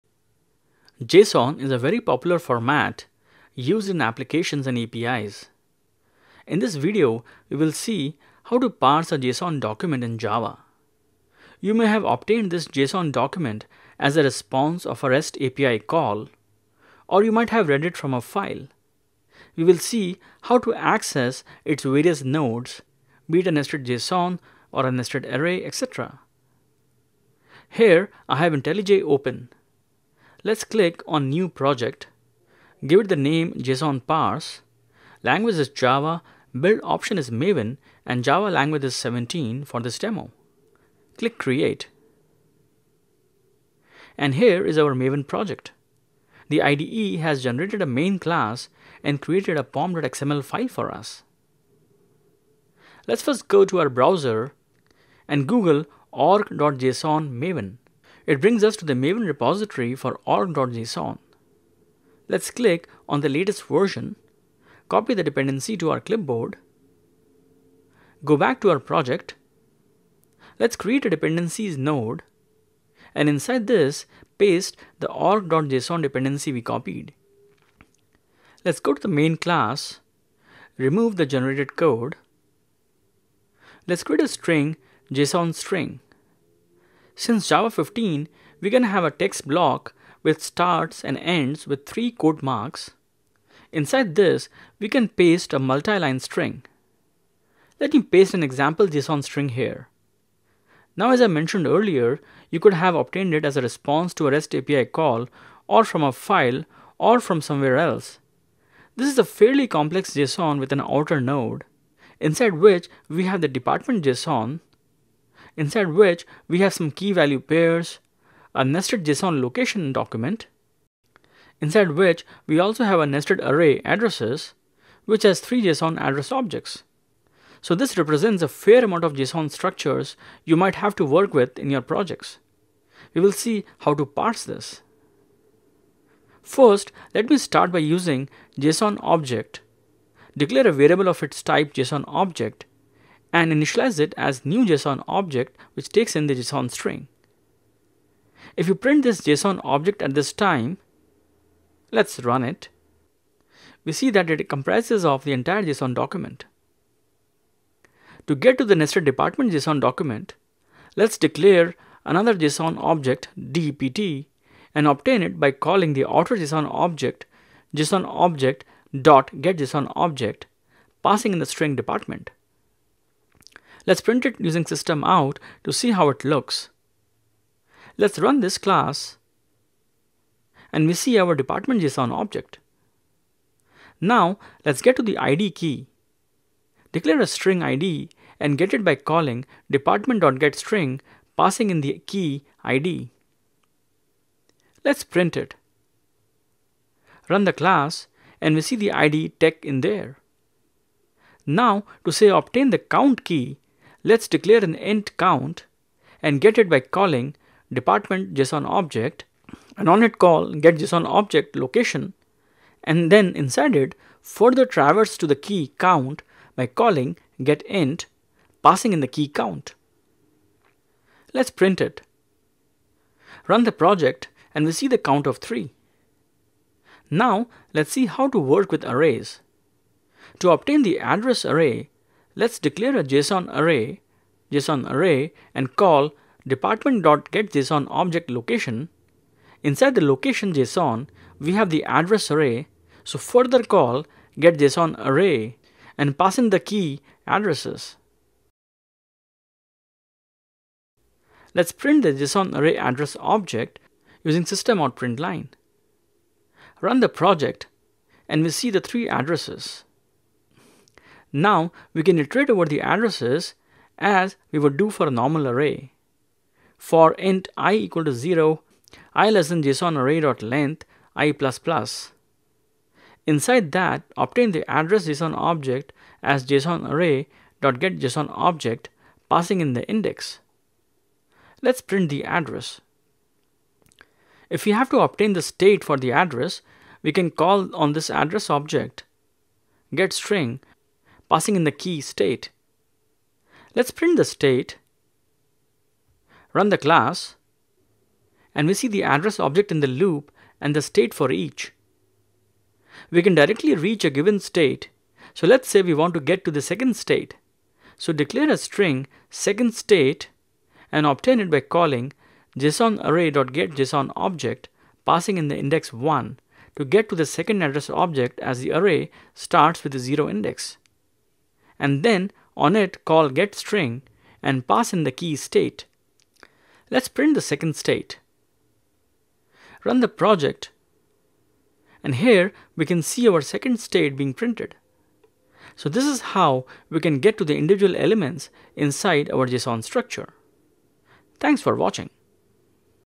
Parsing JSON in Java
In this video we will see how to parse a json object in java, traversing it and getting values for nested jsons, arrays and keys.
You can find the source code for this video at the following github url: